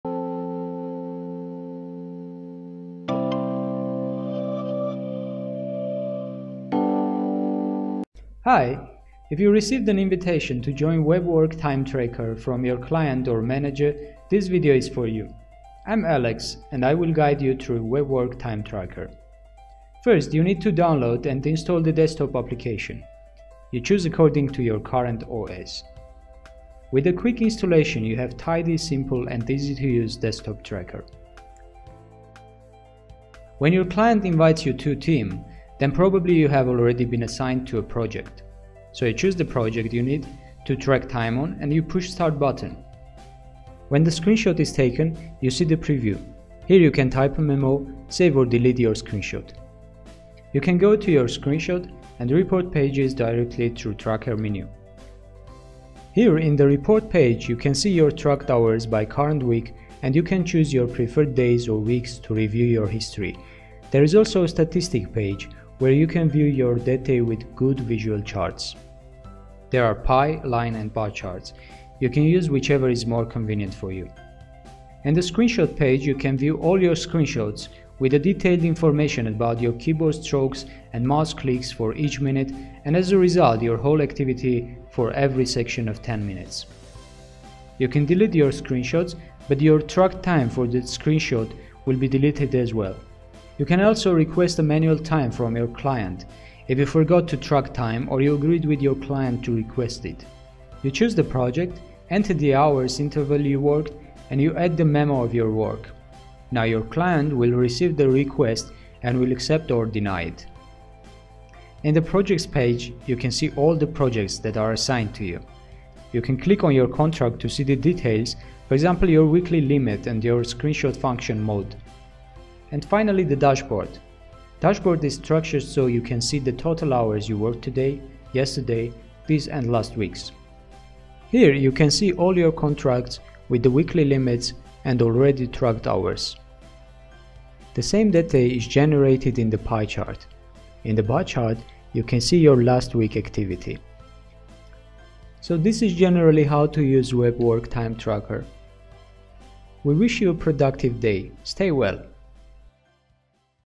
WebWork Tracker Tutorial For Employees
If you are invited to join WebWork Time Tracker from your client or manager, then this is for you